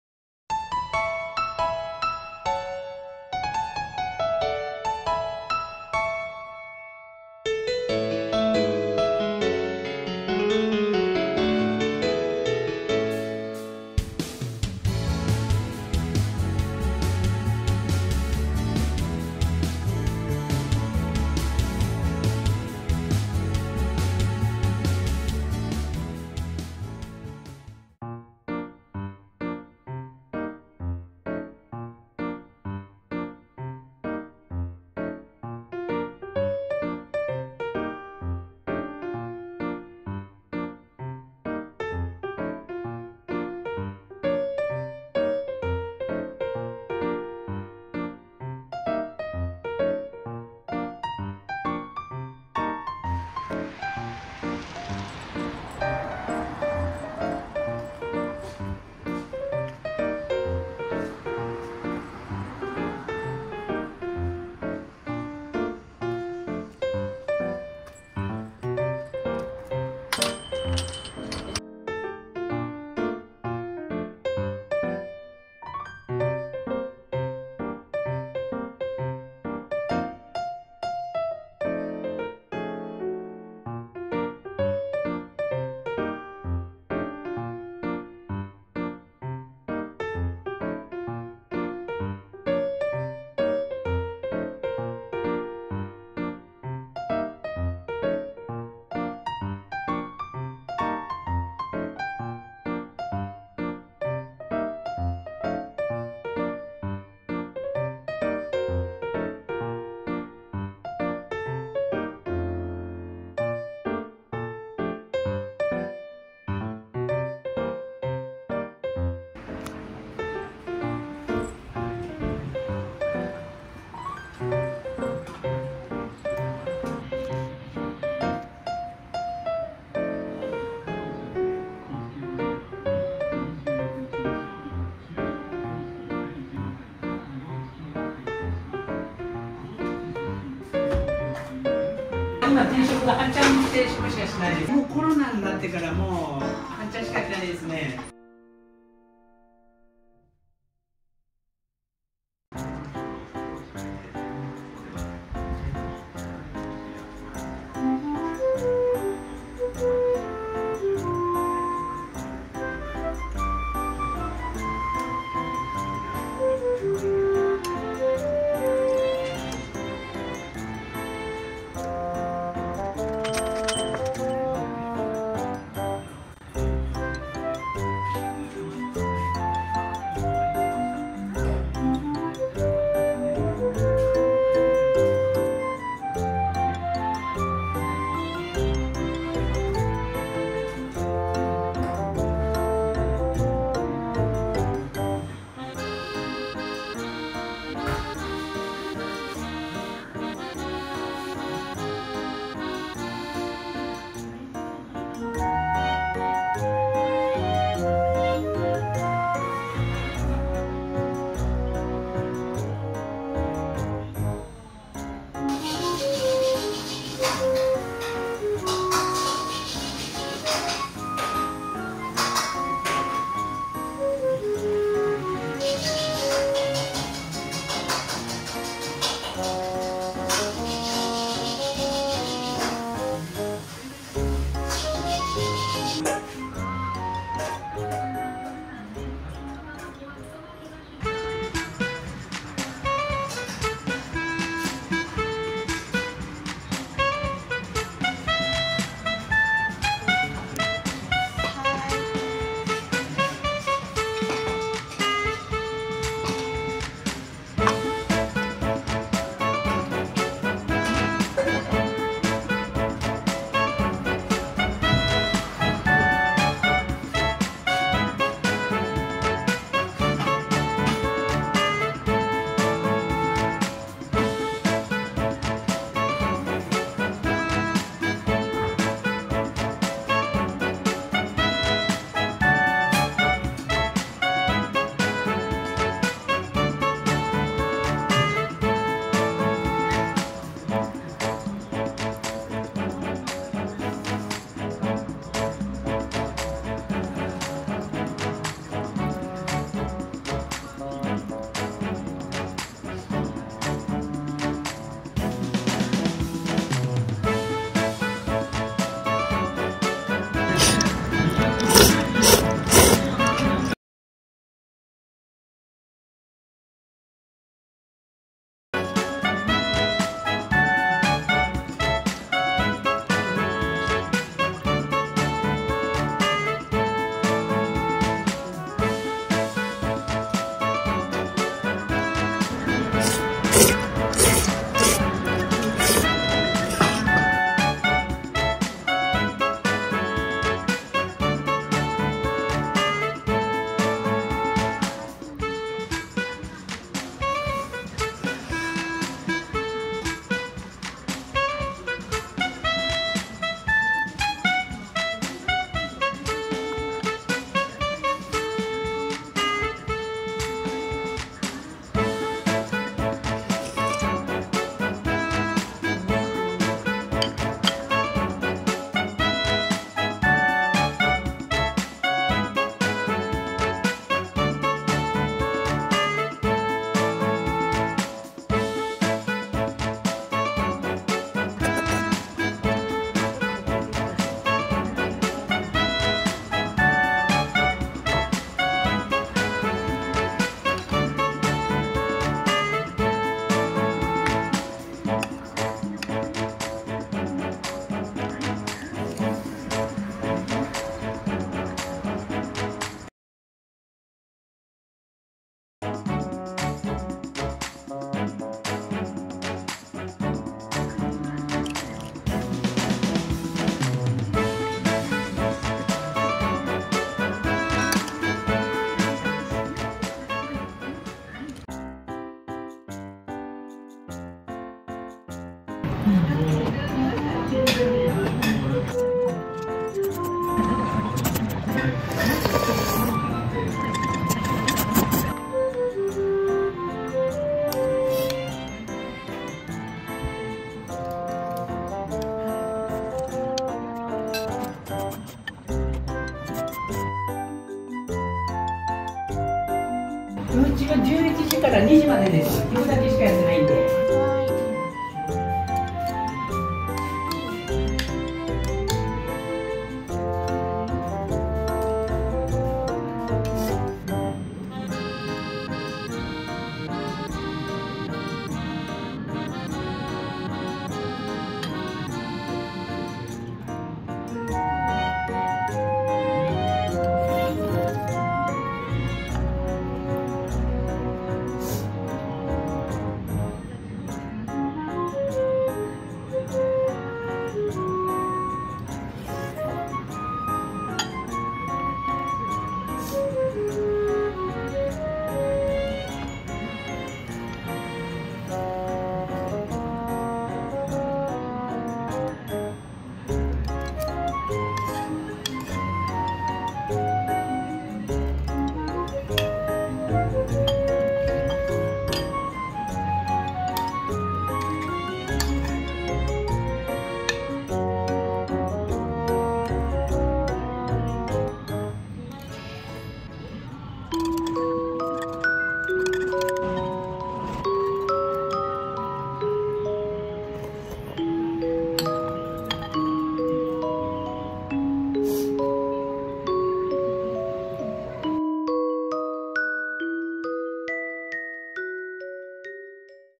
【高知県四万十市】口コミで広がるラーメンの美味さ「麺屋 克」vlog
SNS発信何も無し。電話番号すら載せてない。
人が住んでるとは思えないほどの外観。
それでも広まる旨いラーメンの噂。
ラーメン全種類旨いとの噂。
常連さんに教えて貰ったクセスゴラーメン屋さんです。